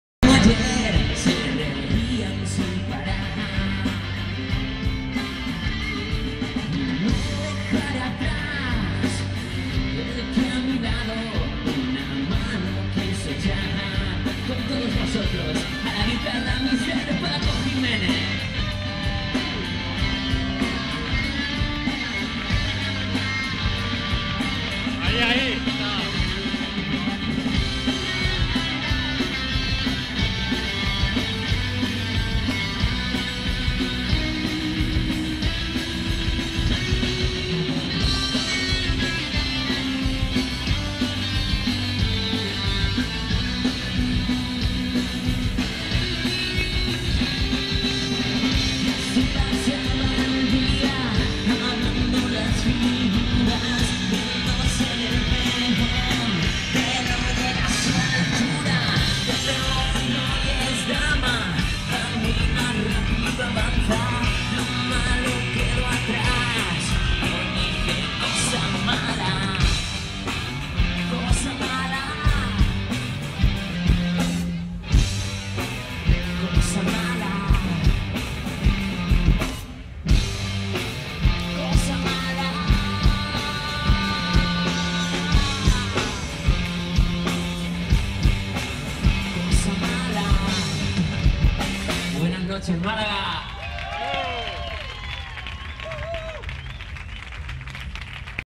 COSA MALA Calle a Calle - Feria de Malaga - Solo de Guitarra (PACO)
COSA MALA en directo en la feria de Malaga el 19 de agosto 09,esta banda malagueña entro todo para sus fan,amigos y todos las personas que participararon de esta gran fiesta.